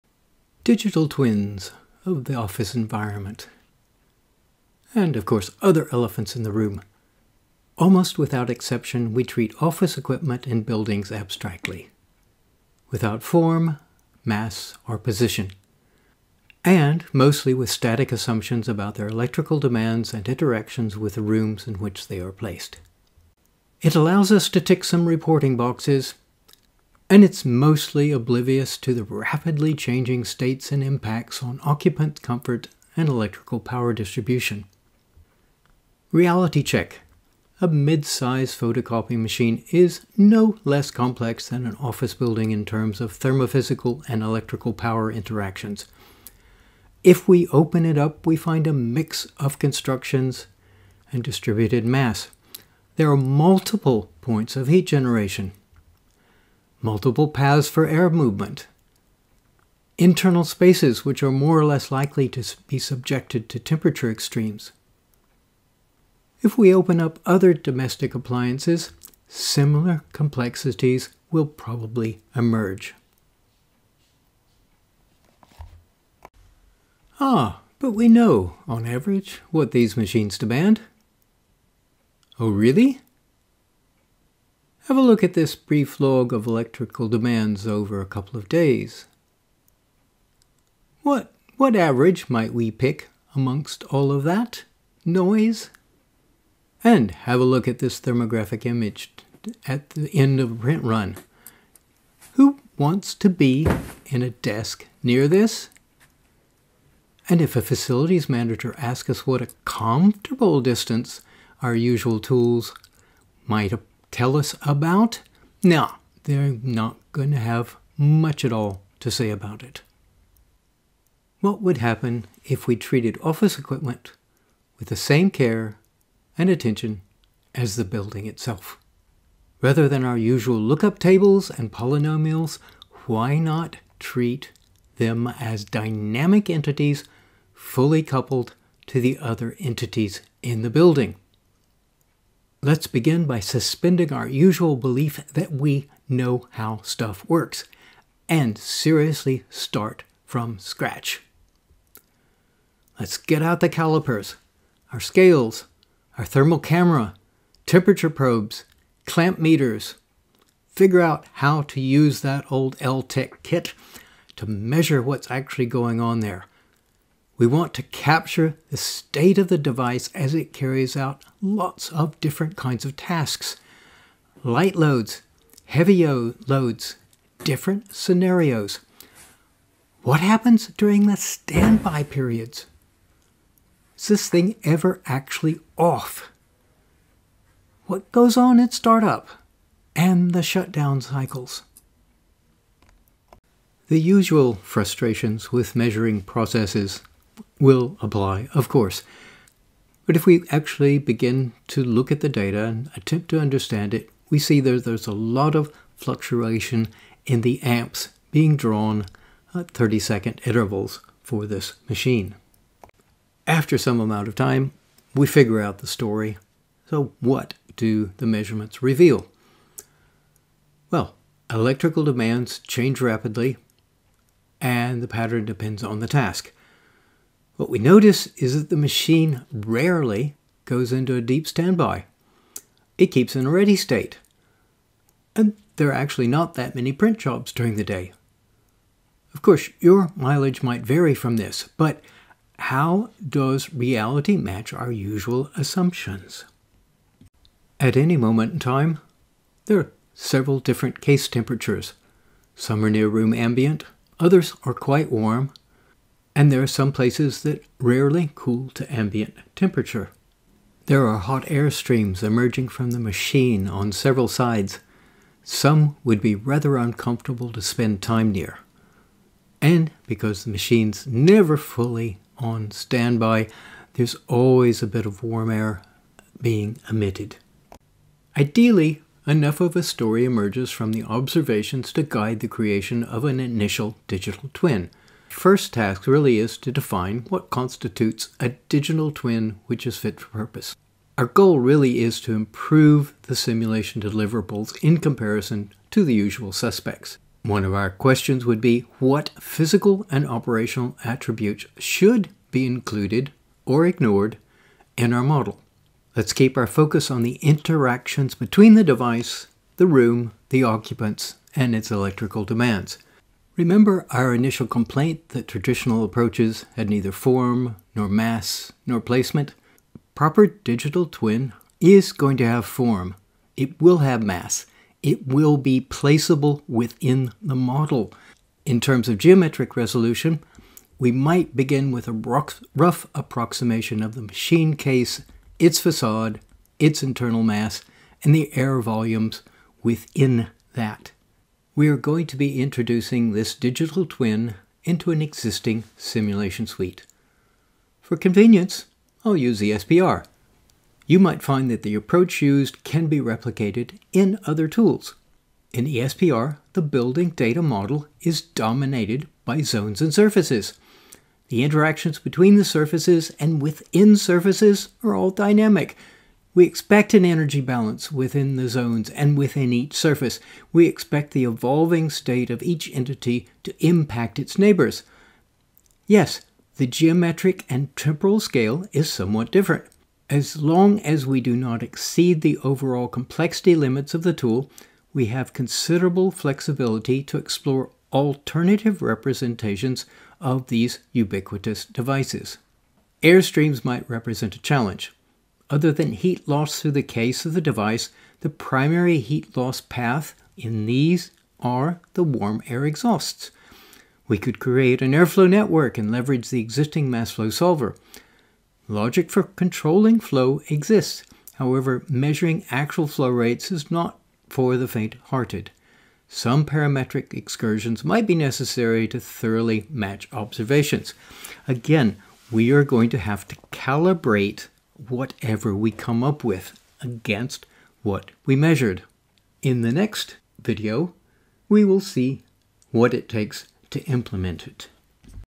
New series: Digital twins of office equipment within whole building simulation.
This video starts a new series focused on the creation of digital twins of office equipment (those sometimes irritating elephants in the room). In this first instalment we start-from-scratch by closely observing their sub-minutely performance. We look for patterns and find they rarely enter a low energy state. We find surface temperatures and exhaust air streams that few staff would want to spend time near - but our tools don’t give us fair warning of this.

We open them up and discover that they are no less complex than the buildings they are installed in. The approaches I (and most simulation teams) use are largely oblivious to this complexity.  Instead of abstractions  without form or mass or position and look-up tables and polynomials in this series we explore options for treating them as dynamic fully coupled entities and leverage the full force of multiple domain solvers.

The next video in the series will explore implementing options - what gets included, what we choose to exclude, levels of detail and how we convert patterns of input power into surface temperatures and air streams that approximate what we observed.